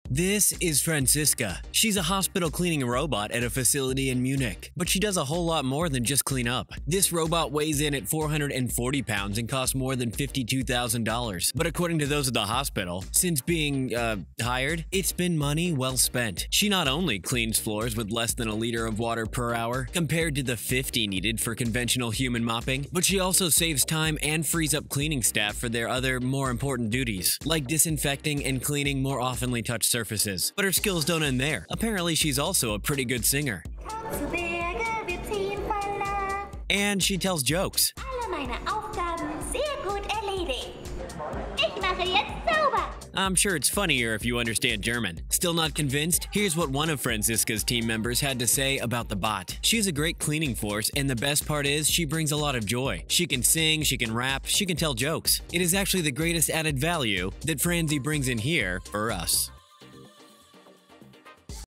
This Robot Sings, Tells Jokes and Cleans at a Hospital in Germany
She’s vying for employee of the month at her hospital. Buzz60’s Tony Spitz has the details.